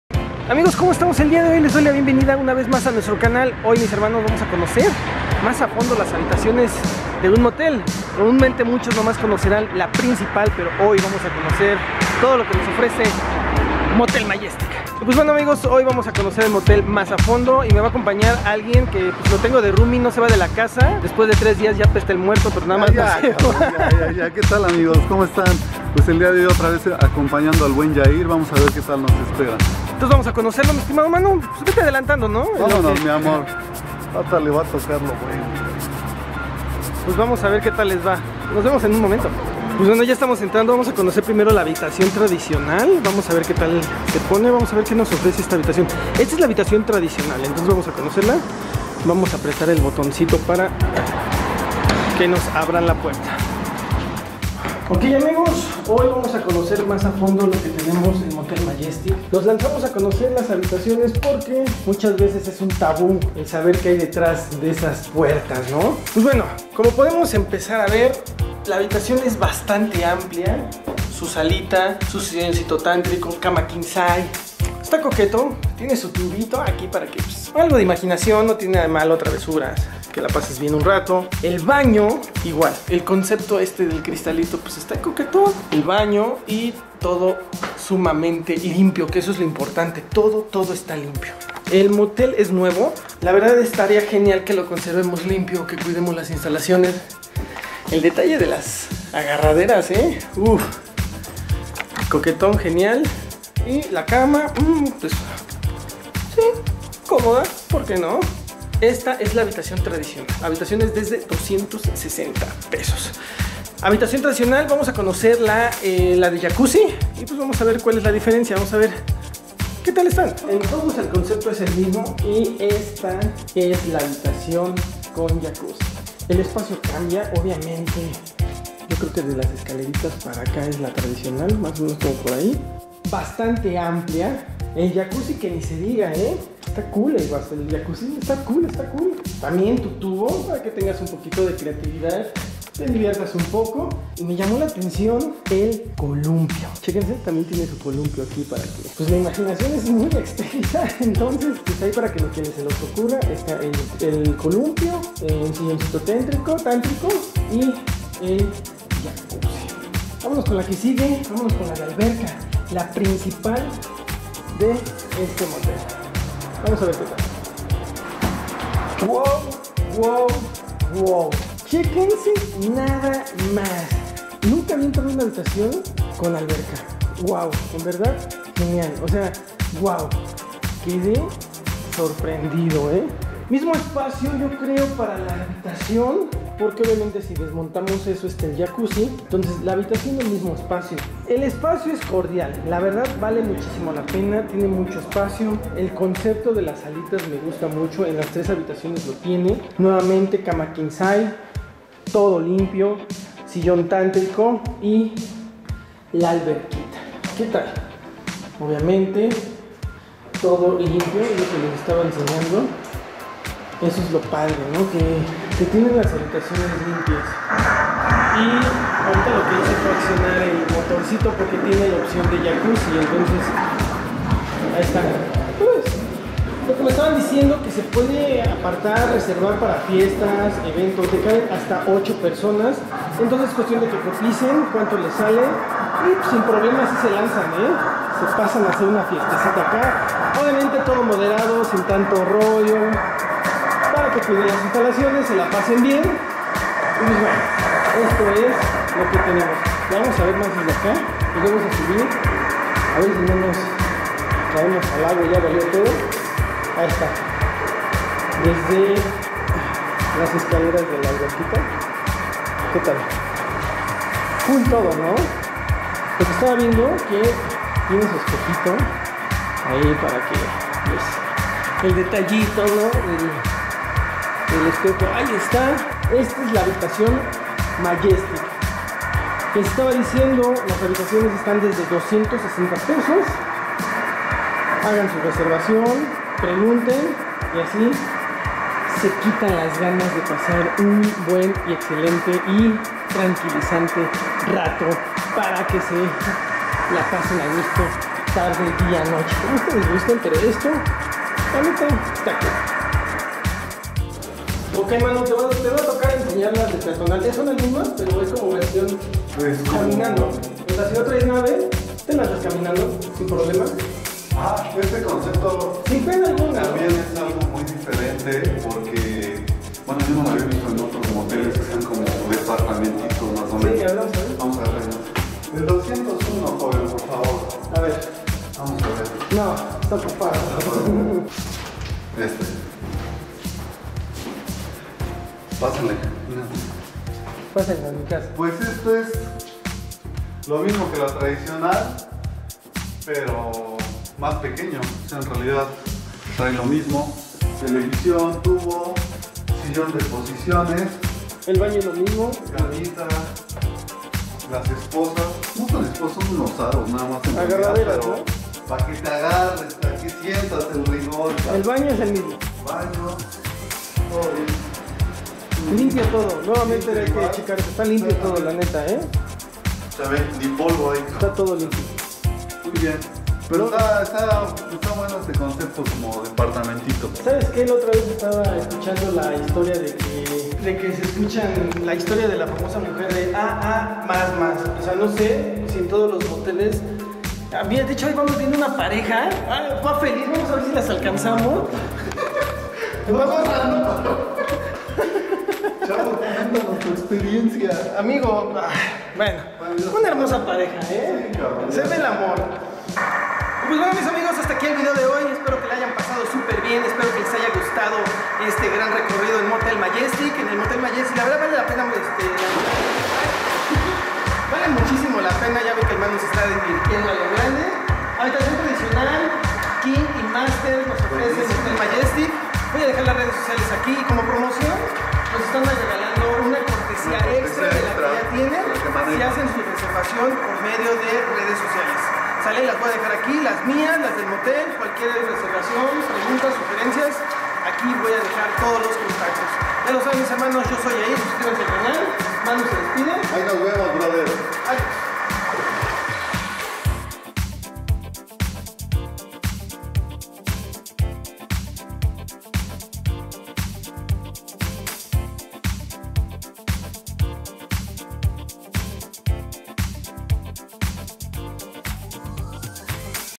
Fui a un Motel nuevo - Visitando el Motel Majestic en puebla
Pues nos escapamos para mostrarles como es por dentro un motel de la ciudad de Puebla

28 sur 128 la paz. A una cuadra de avenida Juárez.

Gracias a las instalaciones por permitir llevar nuestras cámaras a conocer dicho lugar.

Edición: Antao111